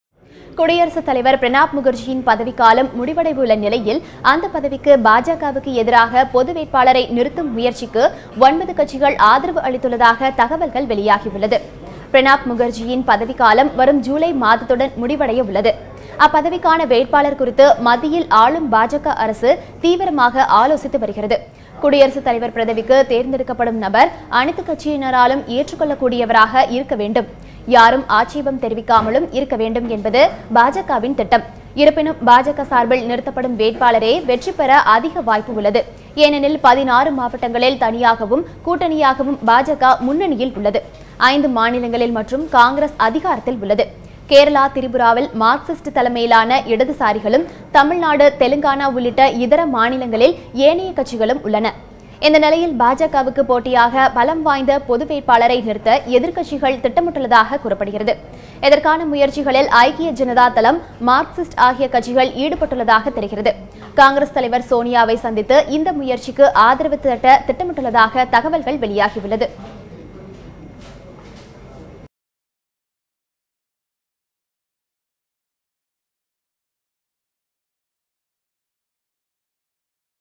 குடியரசுத் தலைவர் பதவிக்கு பாஜகவுக்கு எதிராக பொது வேட்பாளரை நிறுத்தும் முயற்சிக்கு 9 கட்சிகள் ஆதரவு
குடியரசுத் தலைவர் பதவிக்கு பாஜகவுக்கு எதிராக பொது வேட்பாளரை நிறுத்தும் முயற்சிக்கு 9 கட்சிகள்
ஆதரவு